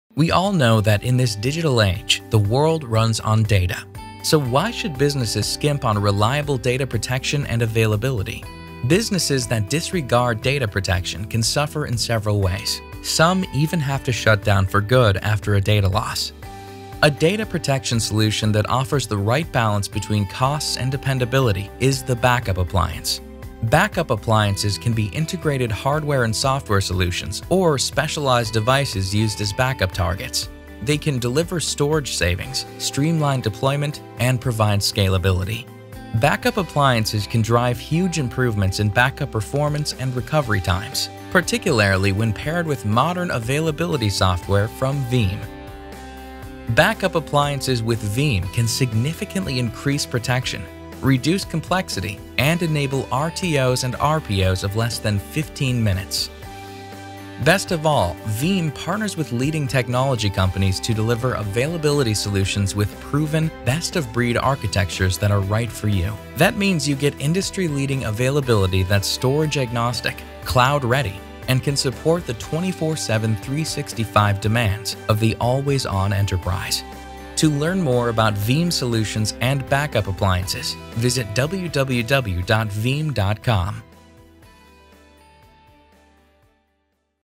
Delivering Availability with Backup Appliances
When used together with our Alliances backup appliances solutions, Veeam® provides the industry's leading combination of secondary storage and Availability software, offering benefits such as faster backup performance, improved RTOs and RPOs, cost savings, ease of use and stronger security. Learn more in this short video.